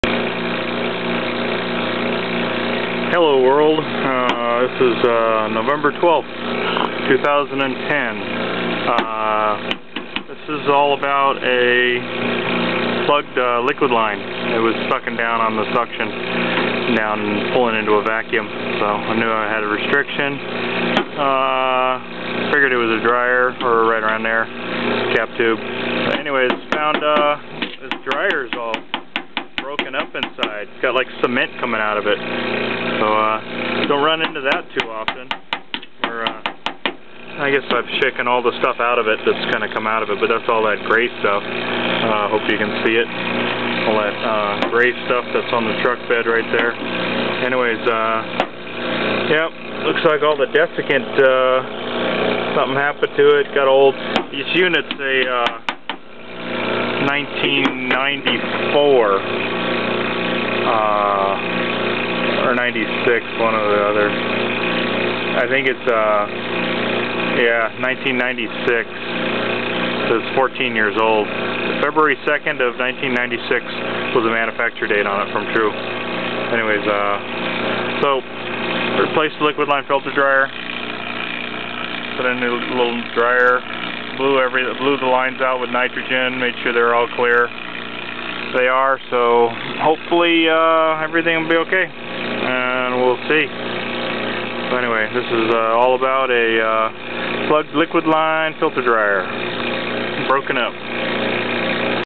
Descintegrated Copper Spun Liquid Line Filter Drier / Plugged.
On a Job Yesterday I Get to a small Sandwhich Prep. Table and See the Suction Pulling Down into a Vacuum add a little Refrigerant no Help Still Pulling into a Vacuum. Well, Sounds like a Restriction in the Liquid line to Me. So, time to open the Refrigeration System and investigate Whether it's just a Liquid drier or the Cap tube is Plugged. Luckilly For me it's Just a Drier that Desintegrated inside and Turned into Concrete Dust. Recovered Refrigerant, Removed Drier to See a buch of Dessicant Dust falling out of it, Dry Nitrogen Blow Backwardds of the Cap. tube to Check for Blockage-Luckilly it's Clear,Install a new Drier,Evacuate Down to 238 microns,Recharge, run and Test. Cleaned Condenser Too with co2 gas.